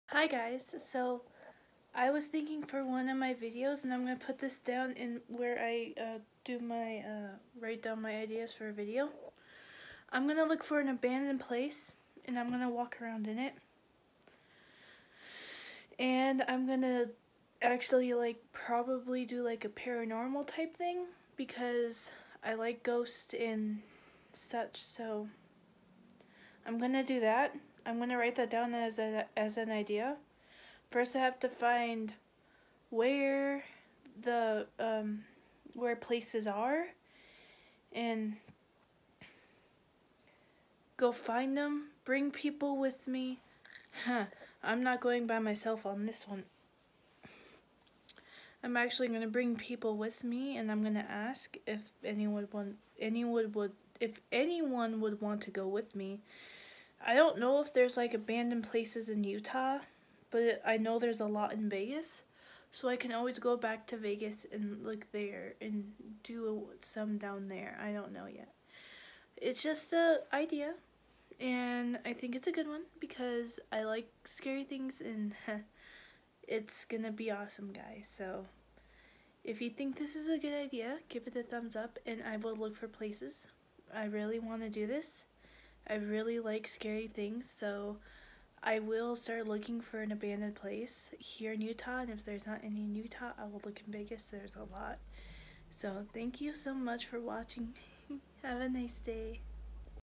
I want to go look through Abandoned places!!!
Yes this is a dream of mine to do this!!! I have always wanted to go into something scary and abandoned!!